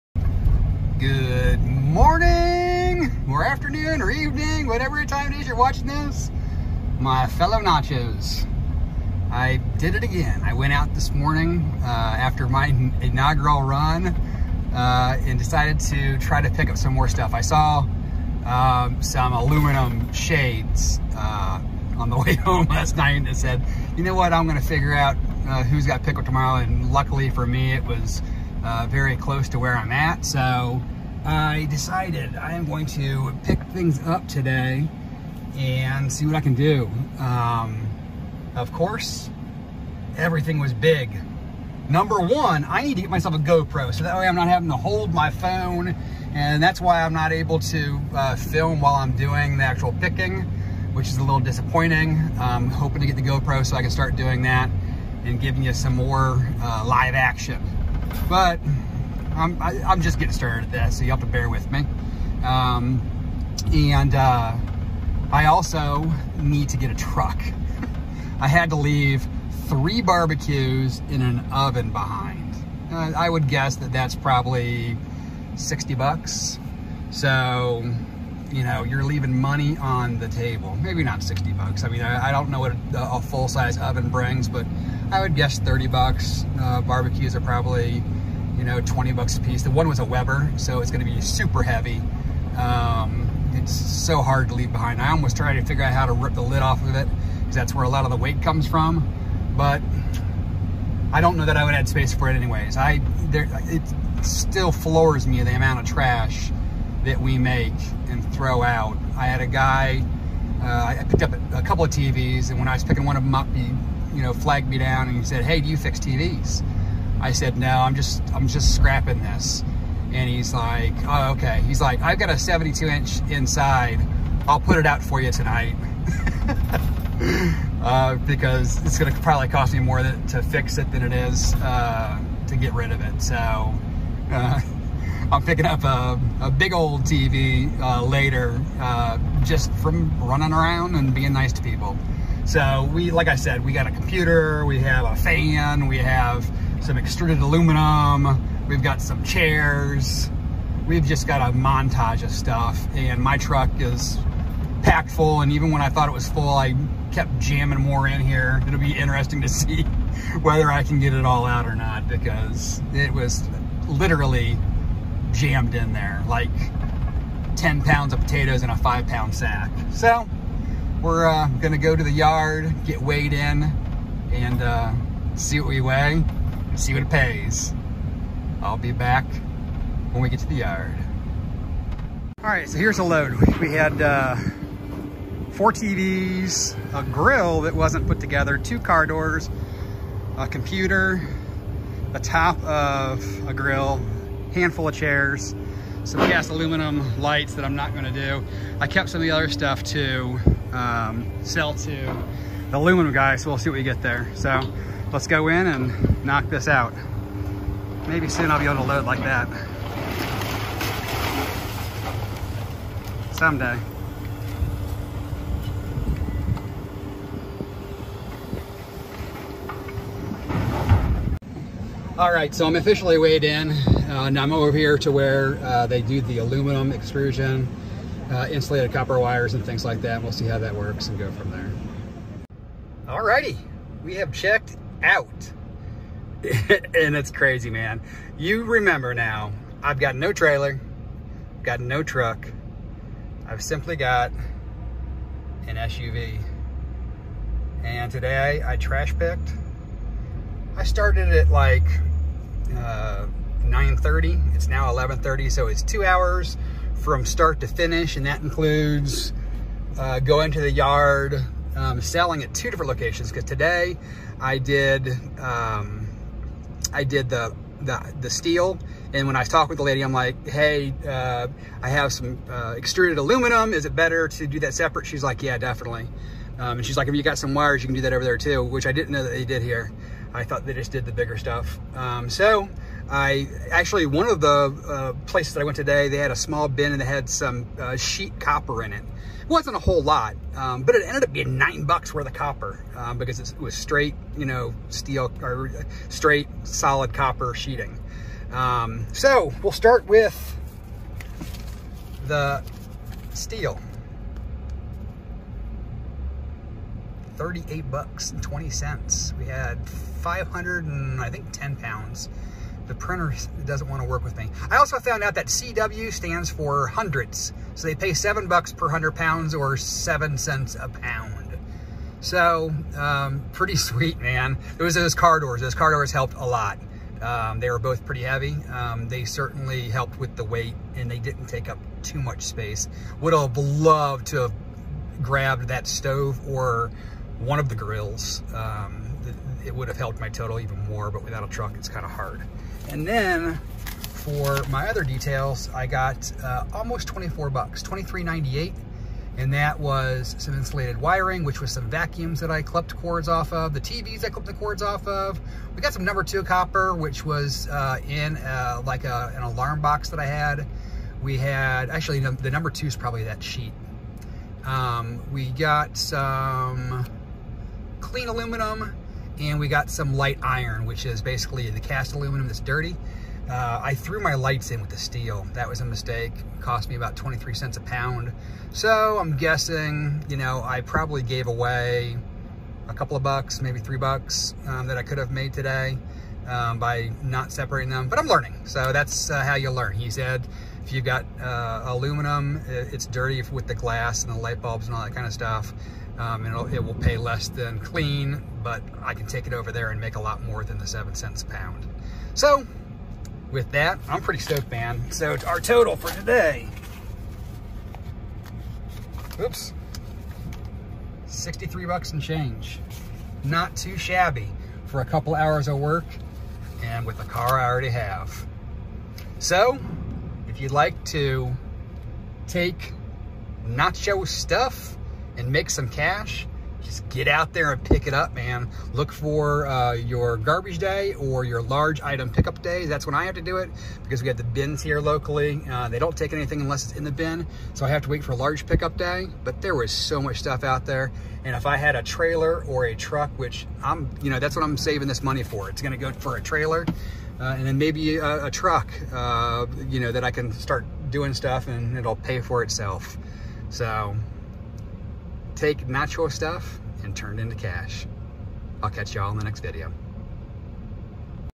Street Scrapping - Make $30+/Hour Without A Truck or Trailer!
So we are on to day two and I am pretty excited.  Stuffed my SUV full and had to even leave stuff behind.  Until I get a truck or trailer, I can not get grills, large appliances and things like that.  It stinks but you work with what you have.  Pretty happy with Day 2 and hope to keep the content coming.  I also hope to get a go pro in the near future.